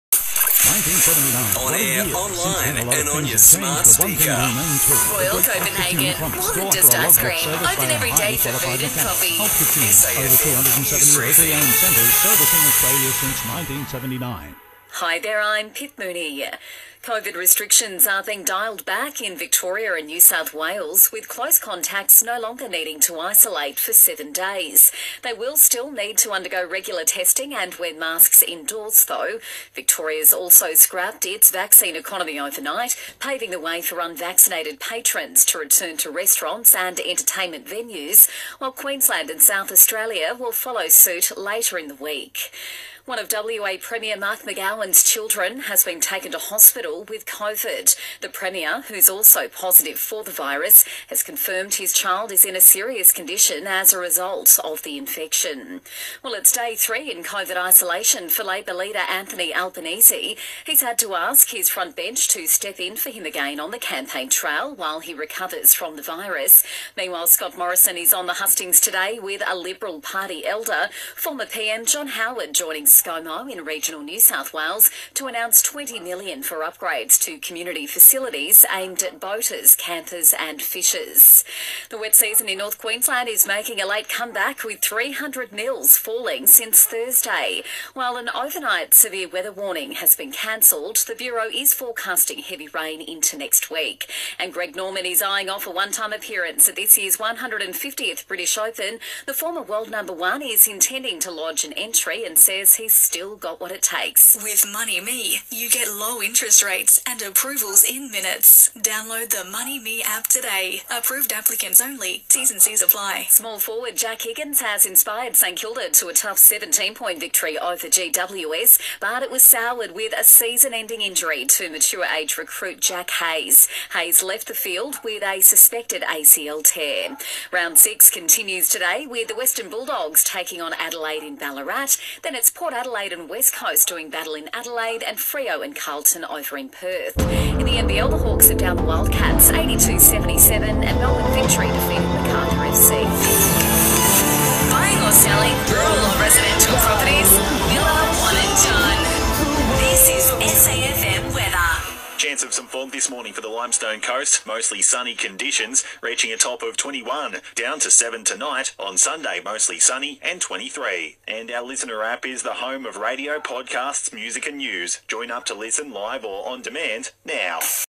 Limestone Coast 96.1SAFM - 11AM Update Sponsored by Ultra Tune, Saturday April 23rd 2022
With Pip Mooney (News and Sport) and Lee Jollo (Weather)